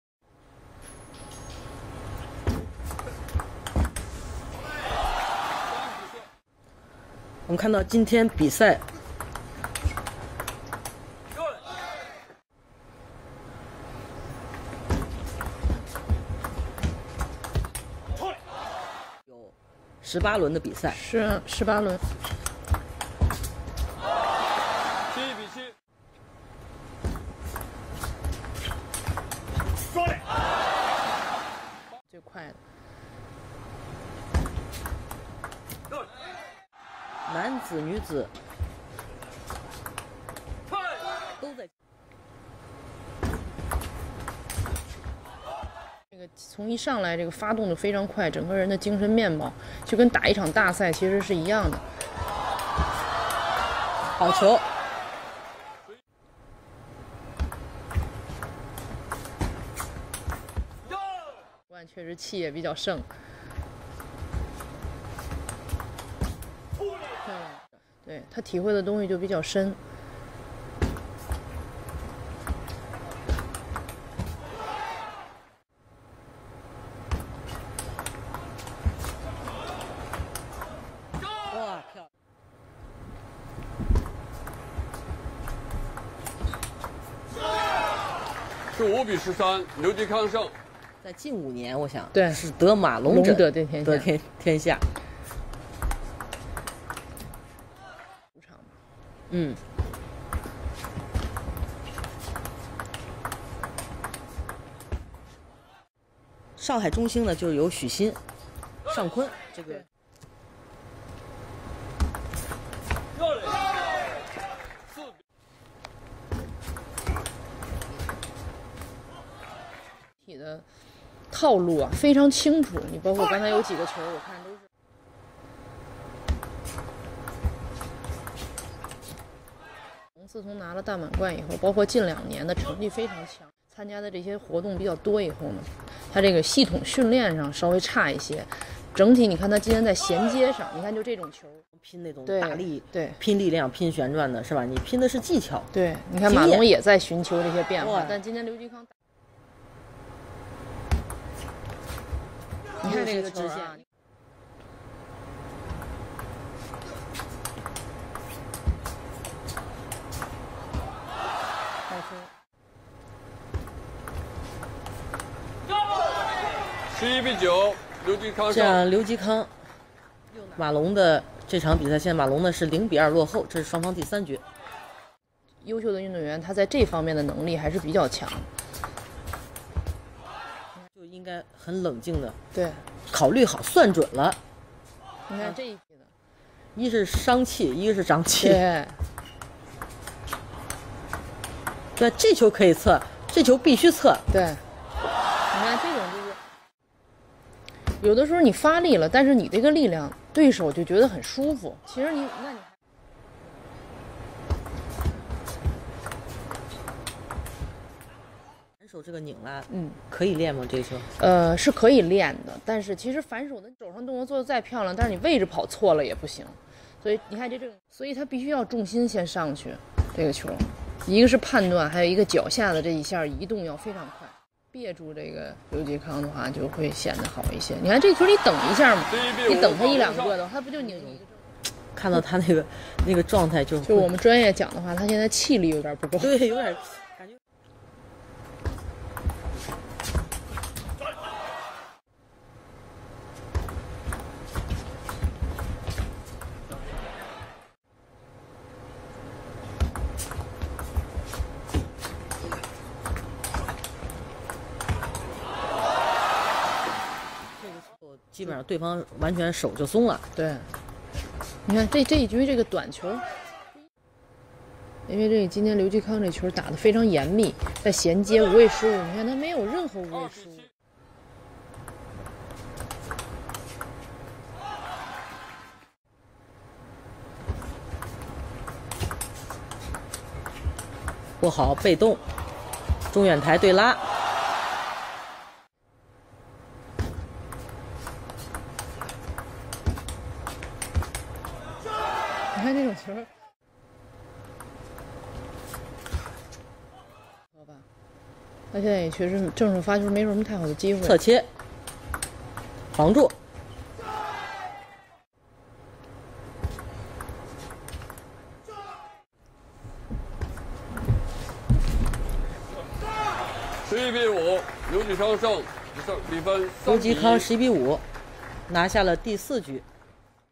Table Tennis Chinese League 2016 - Ma Long Vs Liu Jikang -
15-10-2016
Shandong vs Shenzhen